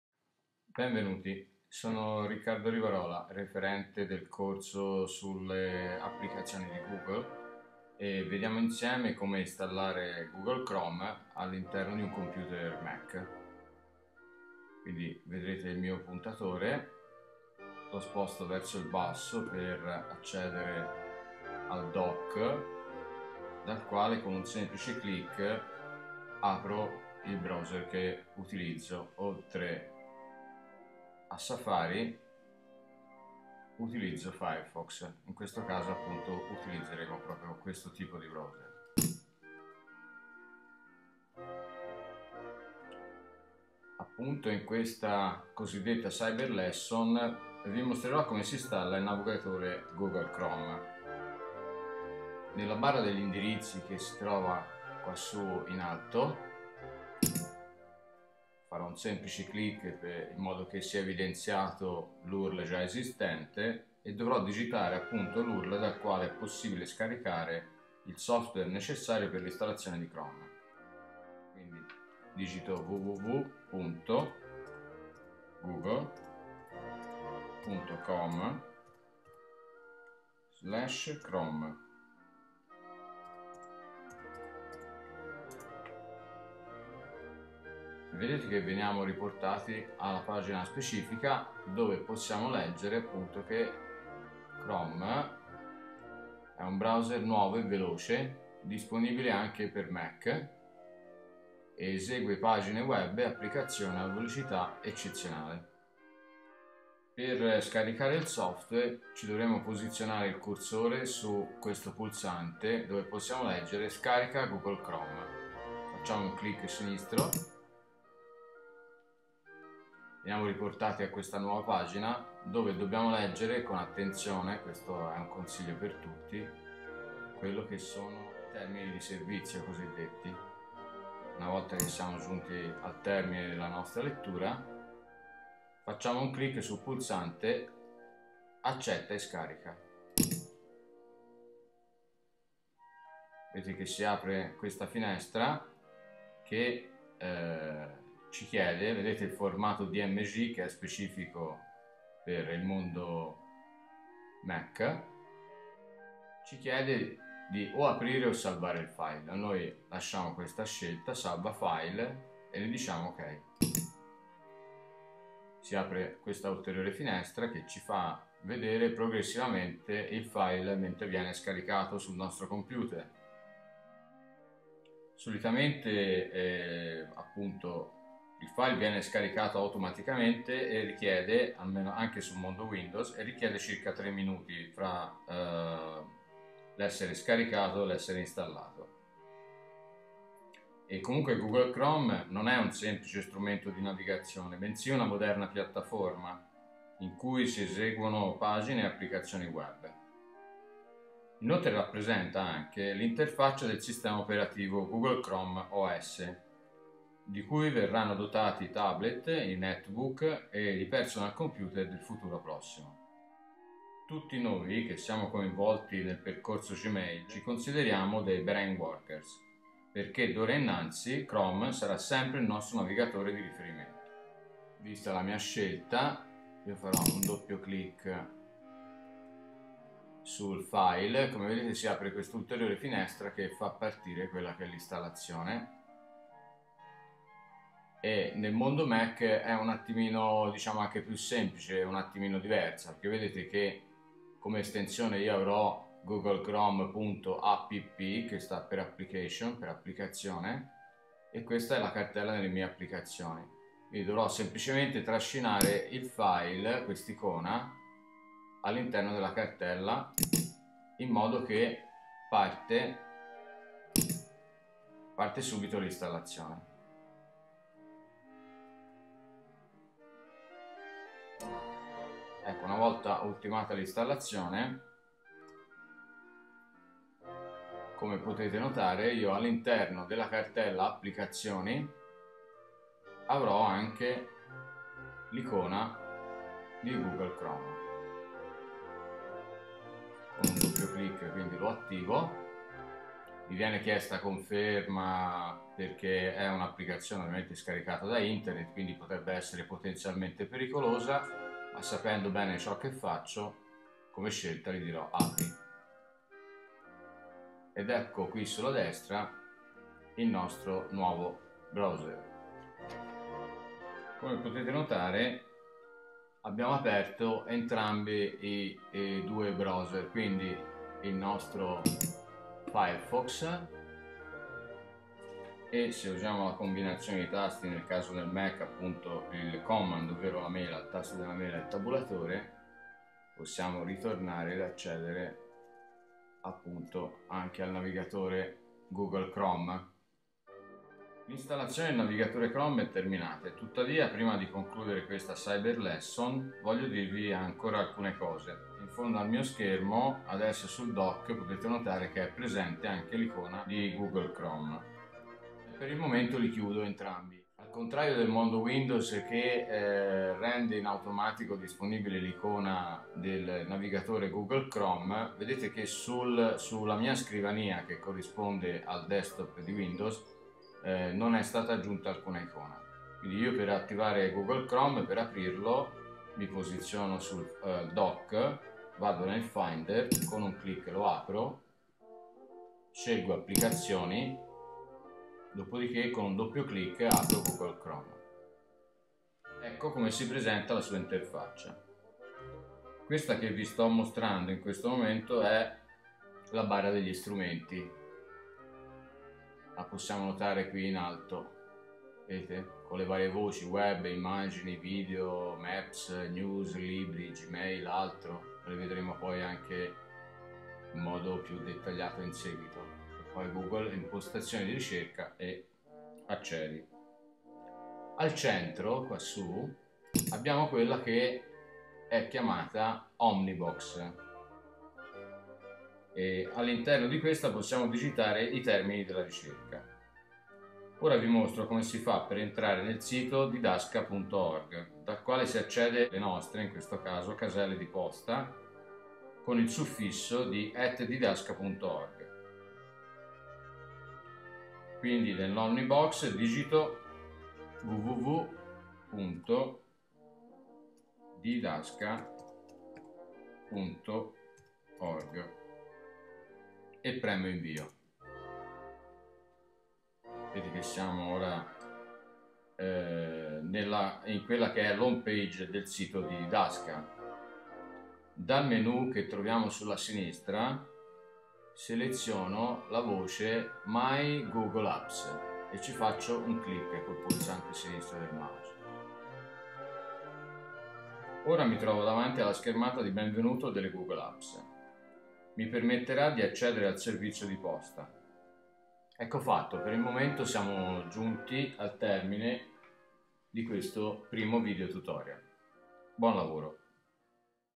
Google_apps_01
Vediamo insieme come installare il browser Google Chrome...